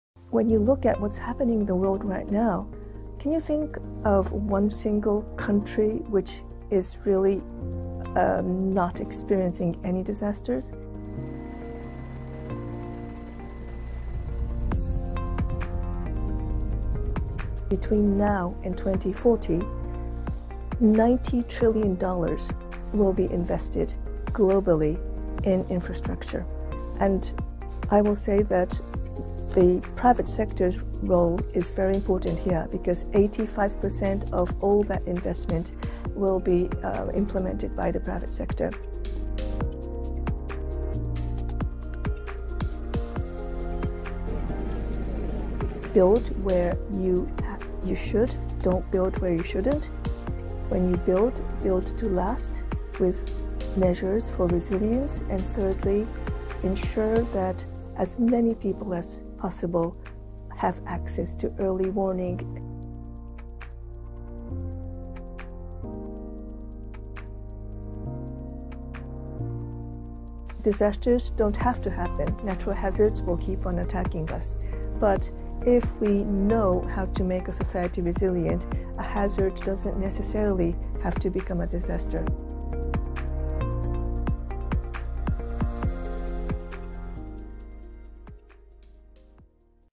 自然ハザードを災害にしないために
自然ハザードを災害にしてはいけません。レジリエント（強靭）なインフラへの投資を通じて、世界は地震、津波や異常気象などにより生じる惨状からコミュニティーと人々を守ることができるのです。水鳥 真美 国連事務総長特別代表（防災担当）は、世界はいかにしてより安全でより持続可能な未来への備えができるかを訴えます。11月5日の「世界津波の日」は、将来の津波リスクから人々の命や財産を守るために、インフラの強靭化、早期警報システム、教育へ投資することの価値に対する認識を高める機会となります。

原題：Stop natural hazards from becoming disasters